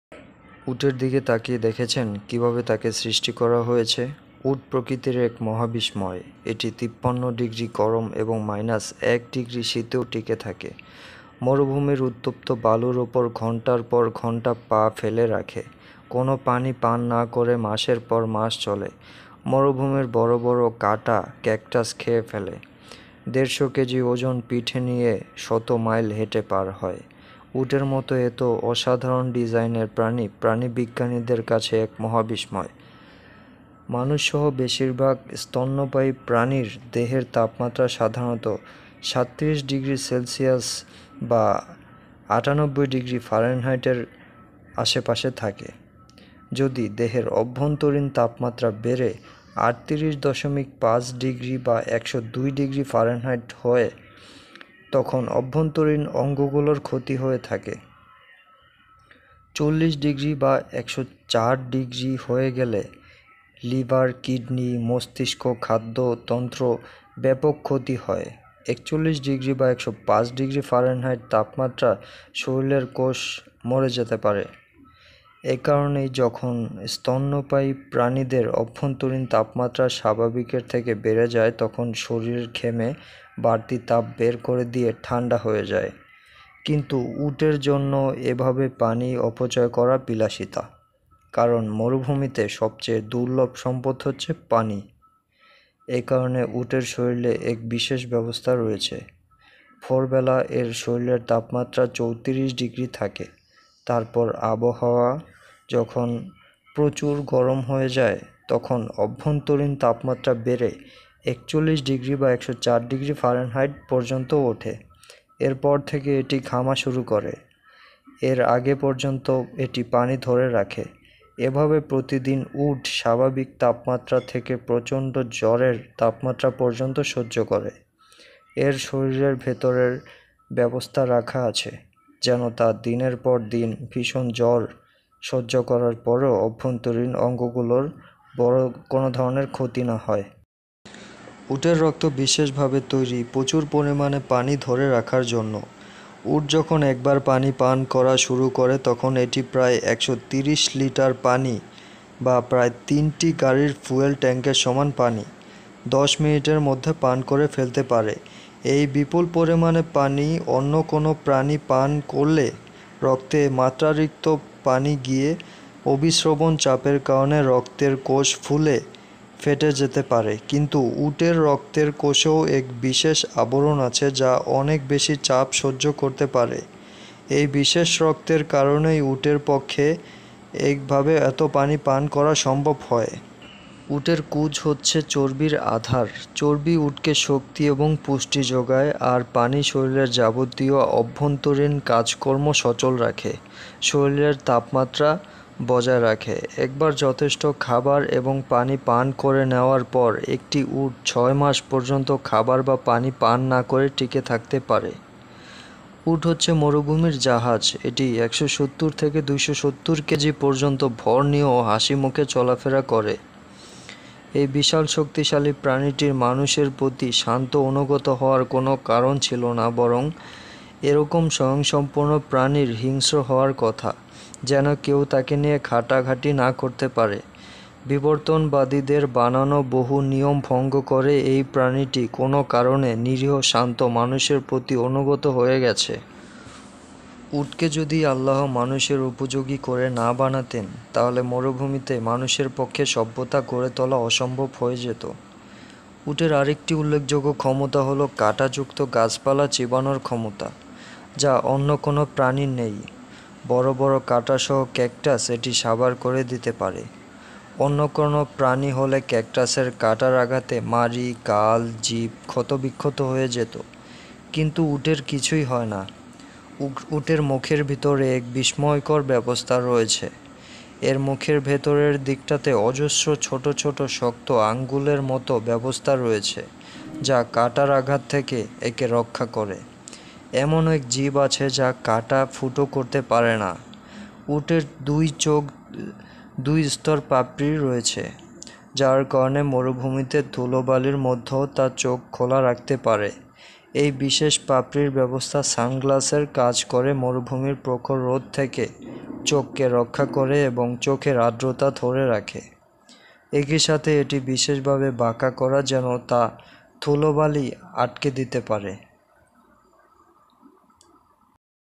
উট সম্পর্কে অজানা কিছু তথ্য, যা জানলে আপনি অবাক হবেন The Story of Life of The Camels
"উটের দিকে তাকিয়ে দেখেছ, কীভাবে তাকে সৃষ্টি করা হয়েছে?” (সূরা গাশিয়াহ ১৭)

উট প্রকৃতির এক মহাবিস্ময়, এটি ৫৩ ডিগ্রি গরম এবং মাইনাস-১ ডিগ্রি শীতেও টিকে থাকে। মরুভূমির উত্তপ্ত বালুর উপর ঘণ্টার পর ঘণ্টা পা ফেলে রাখে। কোনো পানি পান না করে মাসের পর মাস চলে। মরুভূমির বড় বড় কাঁটাসহ ক্যাকটাস খেয়ে ফেলে। দেড়শ কেজি ওজন পিঠে নিয়ে শত মাইল হেঁটে পার হয়। উটের মত এত অসাধারণ ডিজাইনের প্রাণী প্রাণীবিজ্ঞানীদের কাছে এক মহাবিস্ময়।

মানুষসহ বেশিরভাগ স্তন্যপায়ী প্রাণীর দেহের তাপমাত্রা সাধারণত ৩৭ ডিগ্রি সেলসিয়াসের (৯৮ ডিগ্রি ফারেনহাইট) এর আশেপাশে থাকে। যদি দেহের অভ্যন্তরীণ তাপমাত্রা বেড়ে ৩৮.৫ ডিগ্রির (১০২ ফা) বেশি হয়ে যায়, তখন অভ্যন্তরীণ অঙ্গগুলোর ক্ষতি হতে থাকে। ৪০ ডিগ্রির (১০৪ ফা) বেশি হয়ে গেলে লিভার, কিডনি, মস্তিষ্ক, খাদ্যতন্ত্র ব্যাপক ক্ষতি হয়। ৪১ ডিগ্রি (১০৫ ফা) তাপমাত্রায় শরীরের কোষ মরে যেতে শুরু করে।

একারণেই যখন স্তন্যপায়ী প্রাণীদের অভ্যন্তরীণ তাপমাত্রা স্বাভাবিকের থেকে বেড়ে যায়, তখন শরীর ঘেমে বাড়তি তাপ বের করে দিয়ে ঠাণ্ডা হয়ে যায়। কিন্তু উটের জন্য এভাবে পানি অপচয় করা বিলাসিতা। কারণ মরুভূমিতে সবচেয়ে দুর্লভ সম্পদ হচ্ছে পানি। একারণে উটের শরীরে এক বিশেষ ব্যবস্থা রয়েছে। ভোরবেলা এর শরীরের তাপমাত্রা ৩৪ ডিগ্রি থাকে। তারপর আবহাওয়া যখন প্রচণ্ড গরম হয়ে যায়, তখন অভ্যন্তরীণ তাপমাত্রা বেড়ে ৪১ ডিগ্রি (১০৪ ফা) পর্যন্ত ওঠে। এর পর থেকে এটি ঘামা শুরু করে। এর আগে পর্যন্ত এটি পানি ধরে রাখে। এভাবে প্রতিদিন উট স্বাভাবিক তাপমাত্রা থেকে প্রচণ্ড জ্বরের তাপমাত্রা পর্যন্ত সহ্য করে। এর শরীরের ভেতরে ব্যবস্থা রাখা আছে, যেন তা দিনের পর দিন ভীষণ জ্বর সহ্য করার পরেও অভ্যন্তরীণ অঙ্গগুলোর বড় ধরনের ক্ষতি না হয়।

উটের কুজ হচ্ছে চর্বির আধার। চর্বি উটকে শক্তি এবং পুষ্টি যোগায়। আর পানি শরীরের যাবতীয় আভ্যন্তরীণ কাজকর্ম সচল রাখে, শরীরের তাপমাত্রা ঠিক রাখে। একবার যথেষ্ট খাবার এবং পানি নেওয়ার পর একটি উট ছয় মাস পর্যন্ত কোনো খাবার বা পানি পান না করে টিকে থাকতে পারে।

উট হচ্ছে মরুভূমির জাহাজ। এটি ১৭০-২৭০ কেজি পর্যন্ত ভর নিয়েও হাসিমুখে চলাফেরা করে। এই বিশাল, শক্তিশালী প্রাণীটির মানুষের প্রতি শান্ত, অনুগত হওয়ার কোনোই কারণ ছিল না। বরং এরকম স্বয়ংসম্পূর্ণ প্রাণীর হিংস্র হওয়ার কথা, যেন কেউ তাকে ঘাঁটানোর সাহস না করে। বিবর্তনবাদীদের বানানো বহু নিয়ম ভঙ্গ করে এই প্রাণীটি কোনো কারণে নিরীহ, শান্ত, মানুষের প্রতি অনুগত হয়ে গেছে। আল্লাহ যদি উটকে মানুষের জন্য উপযোগী করে না বানাতেন, তাহলে মরুভূমিতে মানুষের পক্ষে সভ্যতা গড়ে তোলা অসম্ভব হয়ে যেত।

উটের আরেকটি উল্লেখযোগ্য ক্ষমতা হলো কাটা যুক্ত গাছপালা চিবানোর ক্ষমতা, যা অন্য কোনো প্রাণীর নেই। বড় বড় কাঁটাসহ ক্যাকটাস এটি সাবাড় করে দিতে পারে। অন্য কোনো প্রাণী হলে ক্যাকটাসের কাঁটার আঘাতে মাড়ি, গাল, জিভ ক্ষতবিক্ষত হয়ে যেত। কিন্তু উটের কিছুই হয় না। উটের মুখের ভেতরে এক বিস্ময়কর ব্যবস্থা রয়েছে। এর মুখের ভেতরের দিকটাতে অজস্র ছোট ছোট শক্ত আঙ্গুলের মত ব্যবস্থা রয়েছে, যা কাটার আঘাত থেকে একে রক্ষা করে। এমন এক জিভ আছে যা কাঁটা ফুটো করতে পারে না।

উটের চোখে দুই স্তর পাপড়ি রয়েছে। যার কারণে মরুভূমিতে ধূলিঝড়ের মধ্যেও তা চোখ খোলা রাখতে পারে। এই বিশেষ পাপড়ির ব্যবস্থা সানগ্লাসের কাজ করে মরুভূমির প্রখর রোদের থেকে চোখকে রক্ষা করে এবং চোখের আদ্রতা ধরে রাখে। একইসাথে এটি বিশেষভাবে বাঁকা করা যেন তা ধুলোবালি আটকে দিতে পারে।